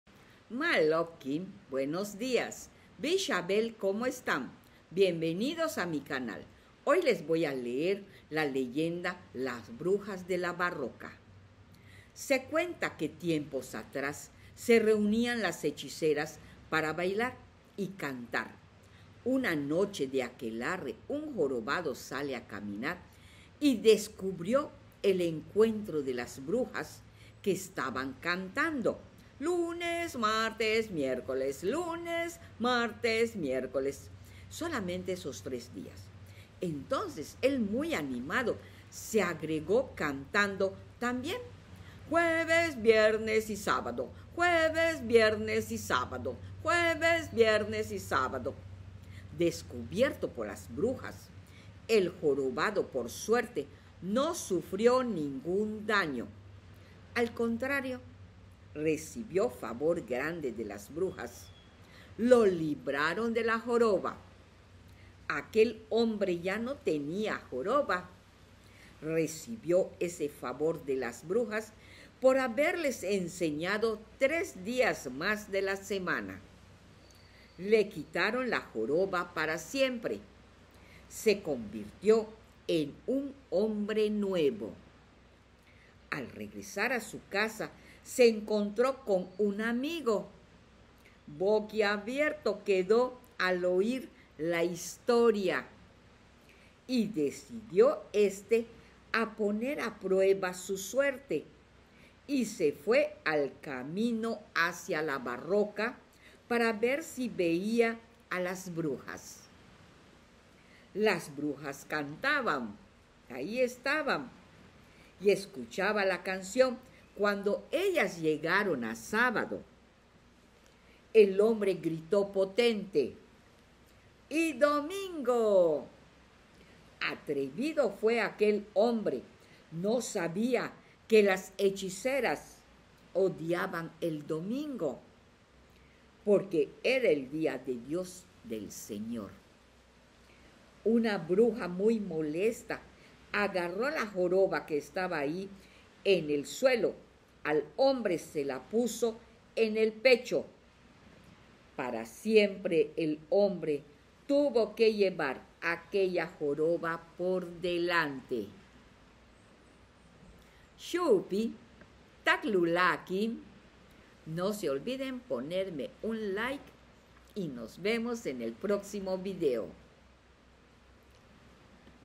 Las brujas de la Barroca
Se cuenta, que las hechiceras se reúnen en noches de luna llena a cantar y bailar para hacer embrujos específicos. Fotos del internet.